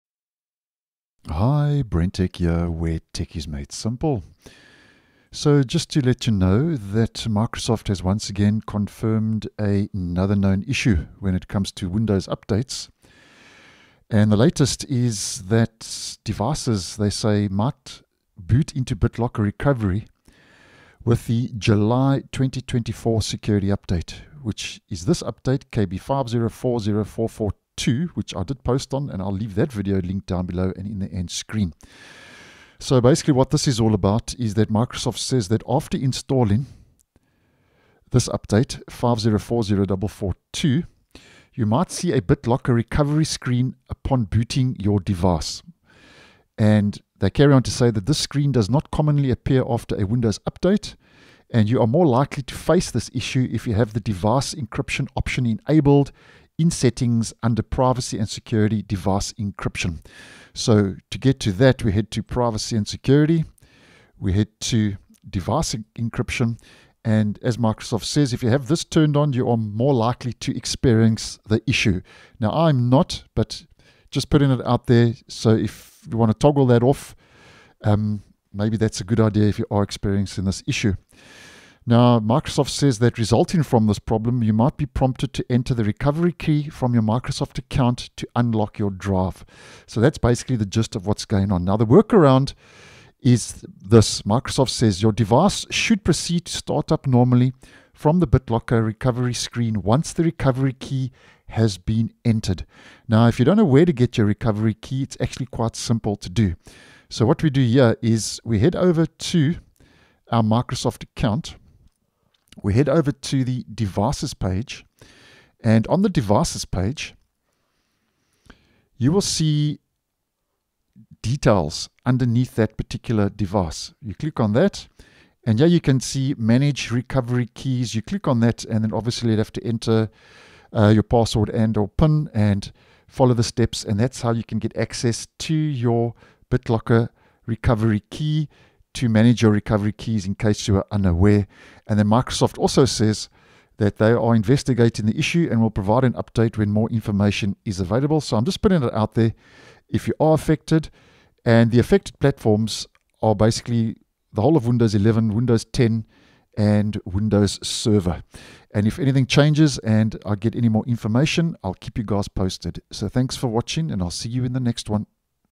Windows 11 KB5040442 is Forcing Devices Into BitLocker Recovery | Workaround
Microsoft has confirmed a new Known issue. After installing the July 2024 Windows security updates, you might see a BitLocker recovery screen upon booting your device.
Windows 11 KB5040442 Released Fixing a Taskbar Known Issue, Includes 6 New Features an "Ad" + More